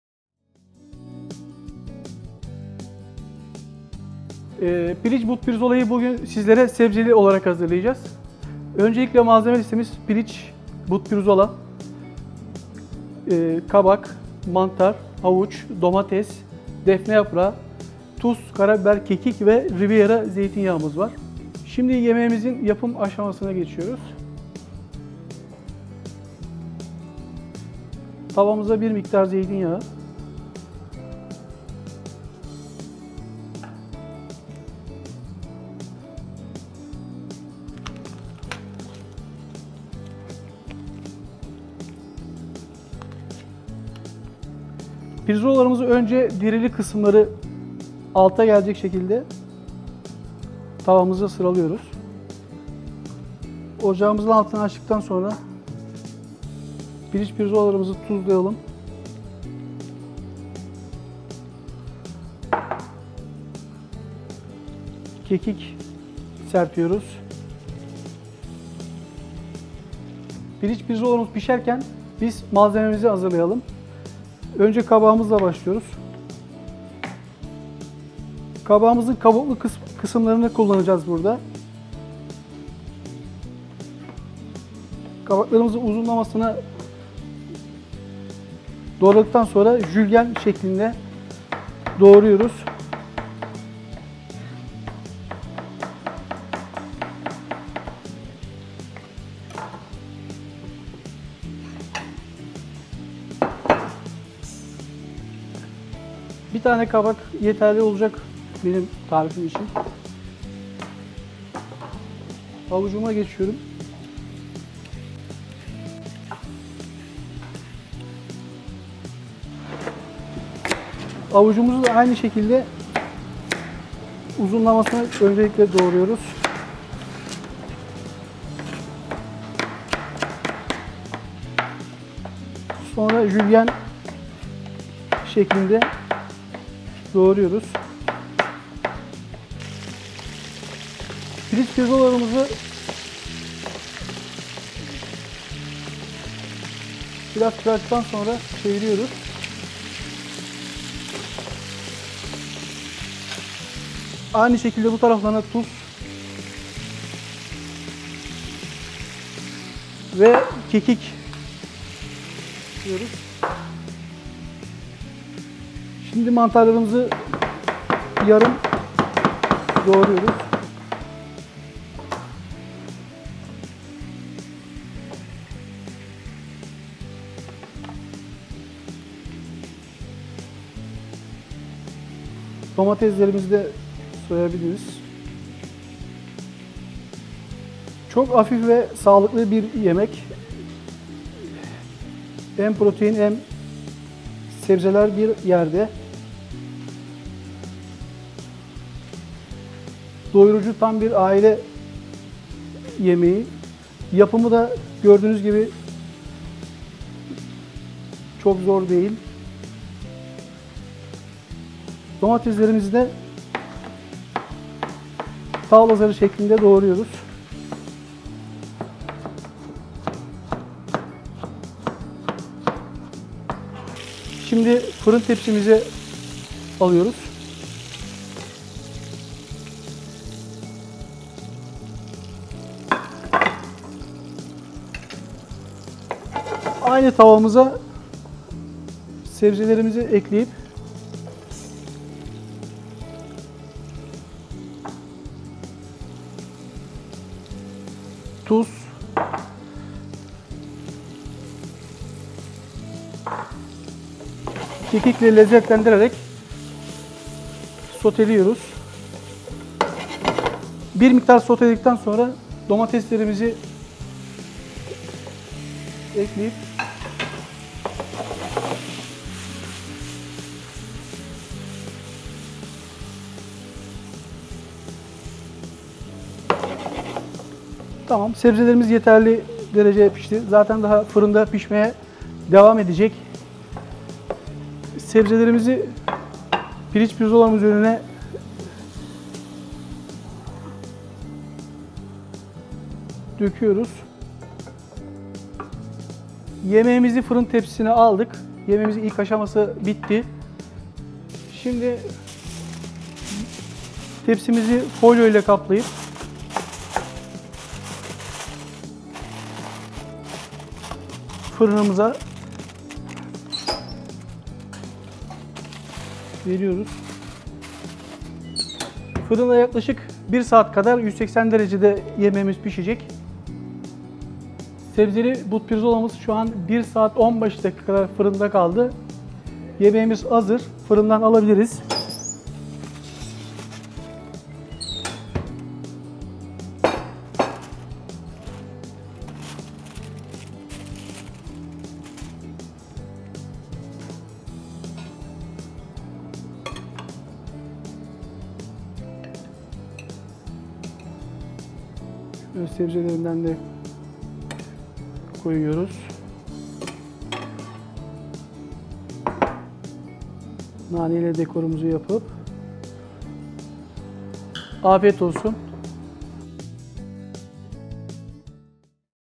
Fırında sebzeli but pirzola nasıl yapılır?
Keskinoğlu Executive Chef Hüseyin Sarıkaş'tan video anlatımlı lezzetli yemek tarifleri.